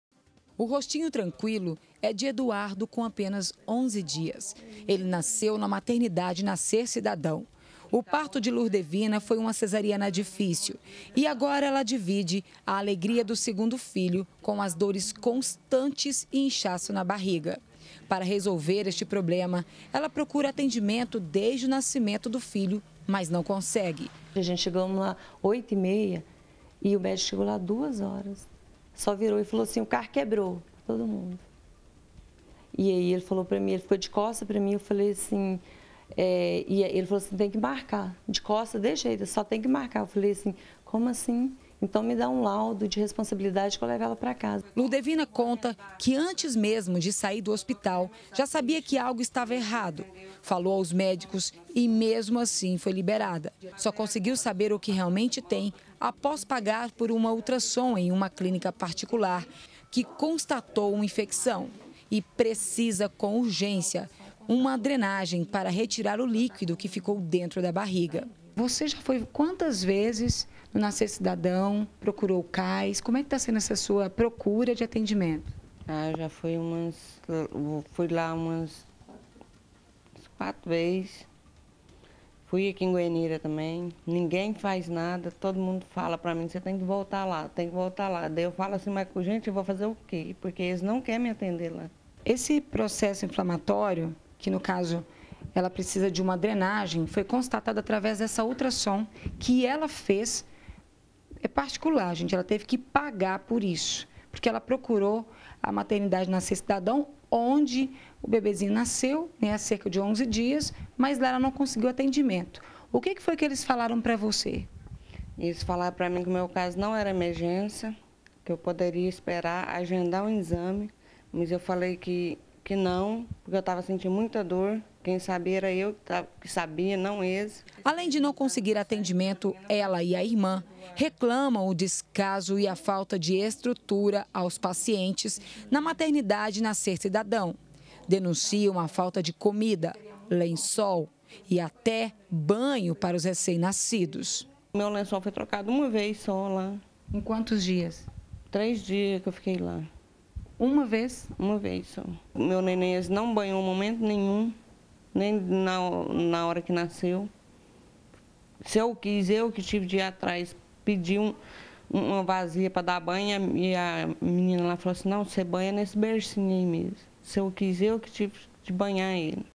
BG - Mulher denuncia descaso em maternidade - 24-04-2018
Mulher denuncia descaso no atendimento na Maternidade Nascer Cidadão. Depois do parto, ela precisa fazer exames e não consegue. Segundo a paciente, faltam materiais básicos de higiene.